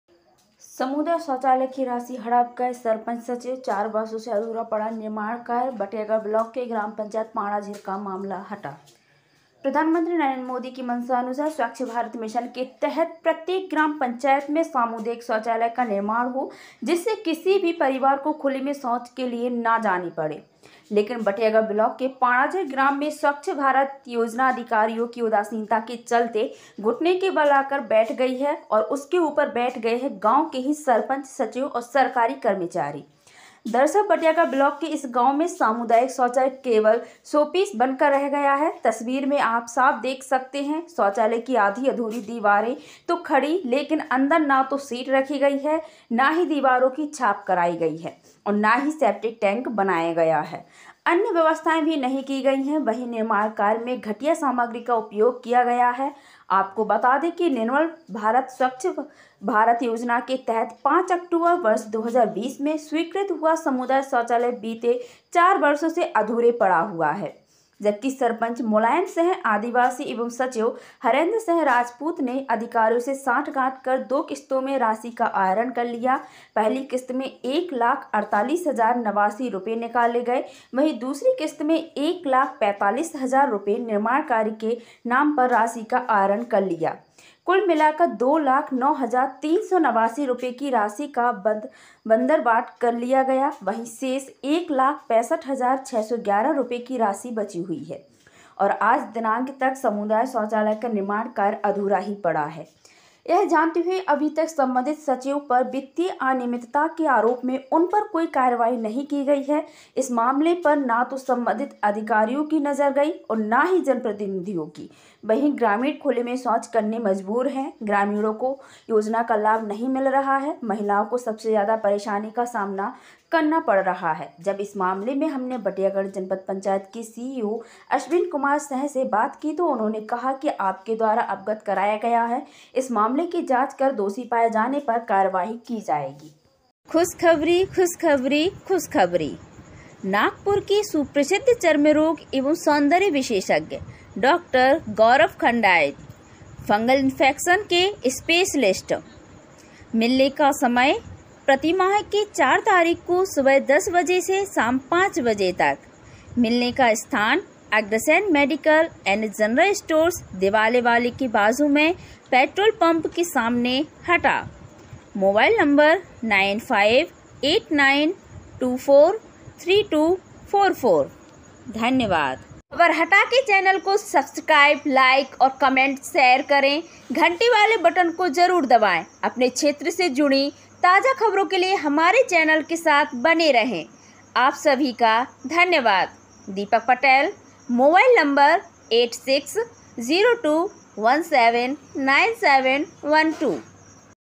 सामुदायिक शौचालय की राशि निकले हो गए चार साल, निर्माण अब भी अधूरा
Deepak.patel
बटियागढ़ ब्लॉक की ग्राम पंचायत पाड़ाझिर का मामला